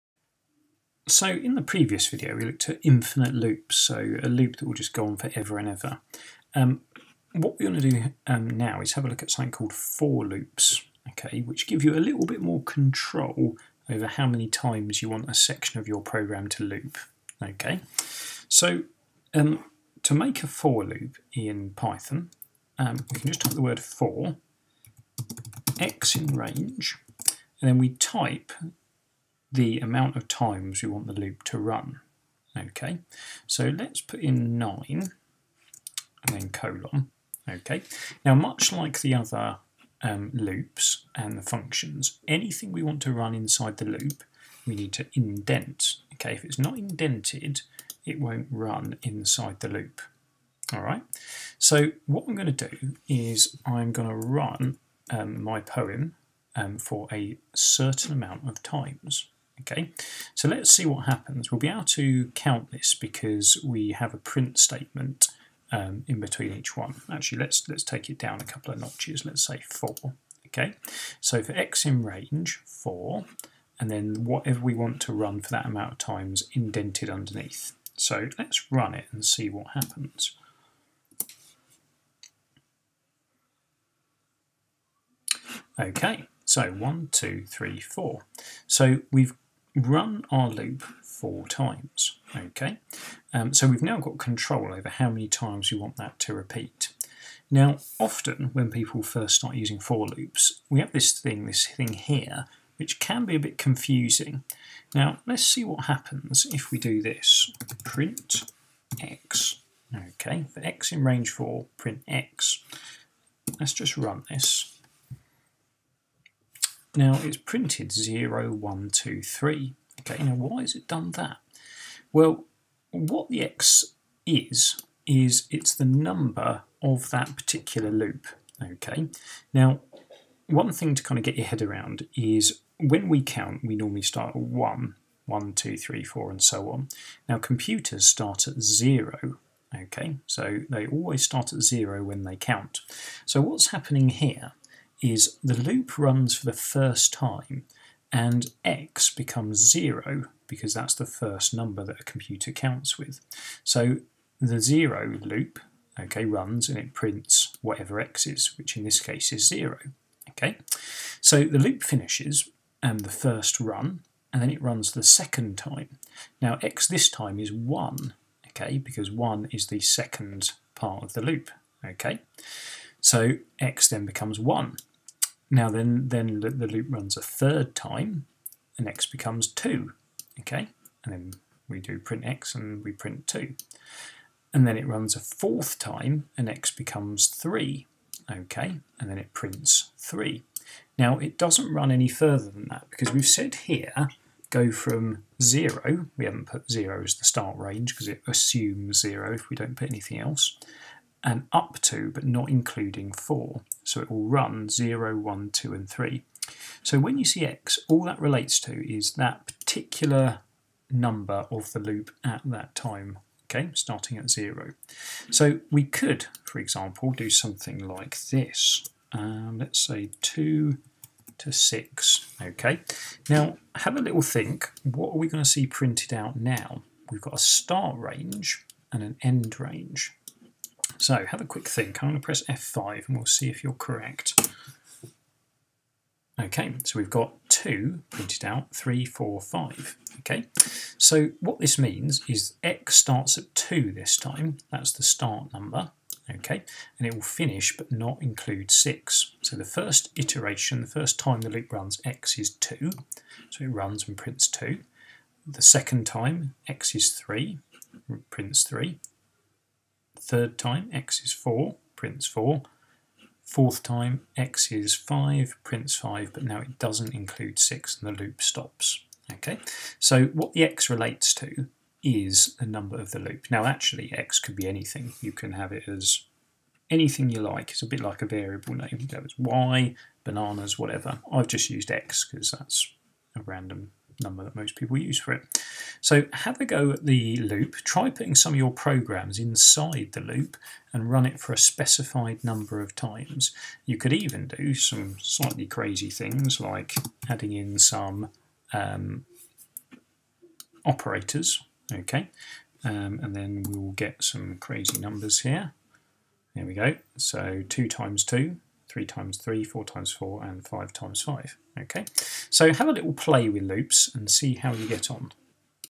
Learn to Code for Beginners 7: For Loops
These videos will teach you the fundamentals of coding and are specifically aimed at complete beginners with no coding experience. All videos are Python based - this can be downloaded for free

This tutorial will show you how to gain control over how amen times you want sections of your code to loop.